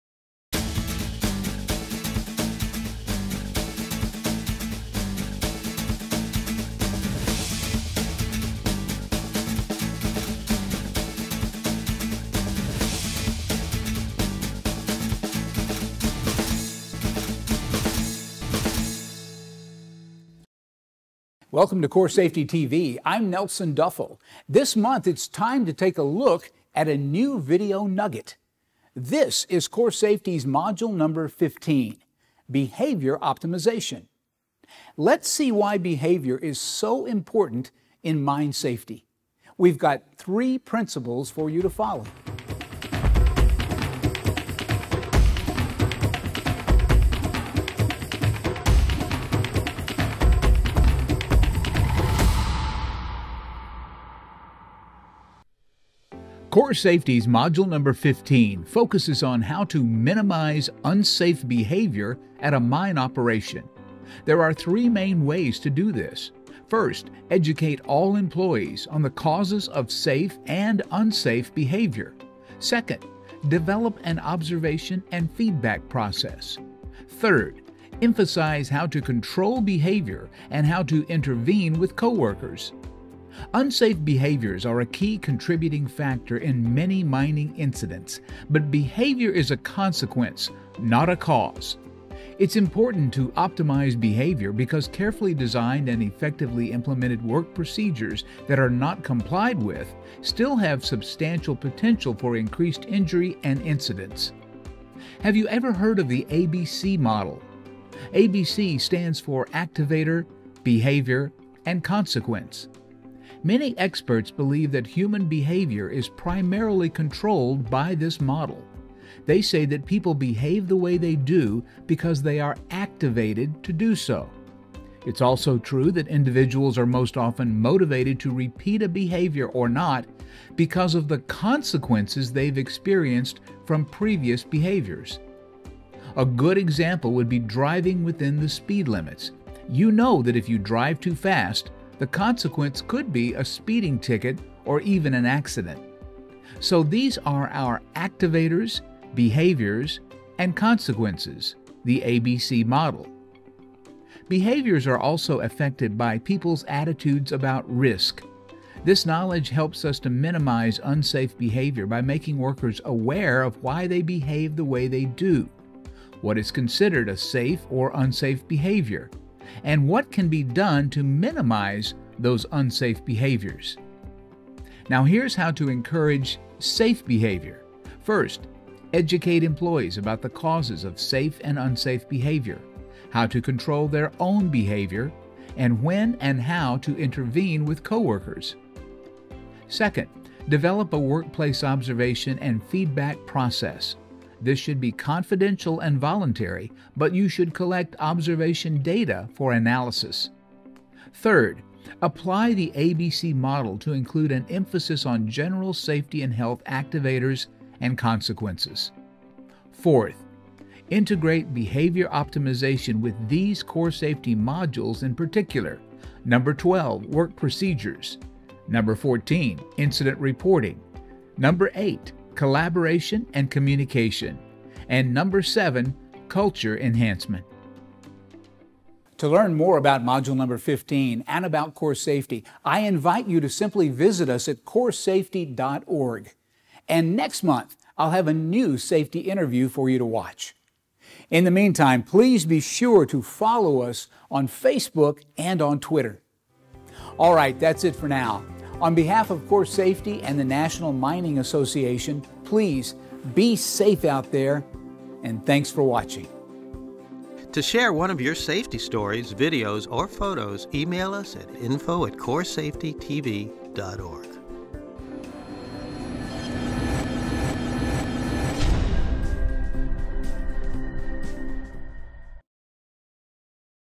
CORESafety TV: August 2023
Welcome to the August edition of CORESafety TV — the online video program from the National Mining Association. This month, we review Module number 15, Behavior Optimization.